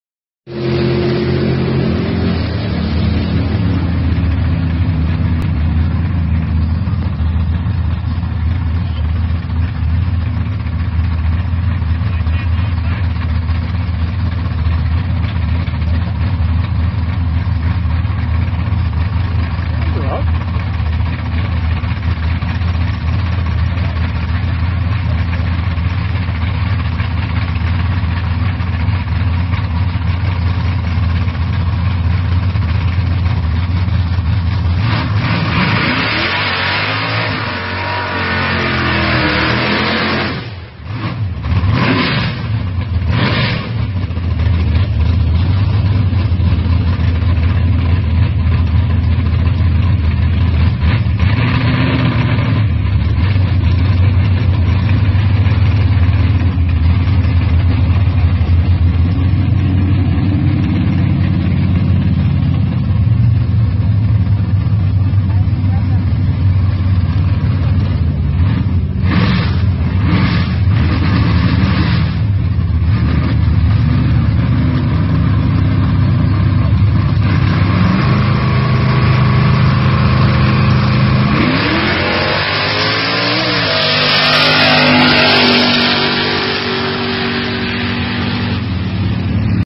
HK GTS, Wilby 1/8th mile
Grants HK GTS, first shakedown run at Wilby 1/8th mile. Driven by Mick from Brody Automotive. HK GTS Wilby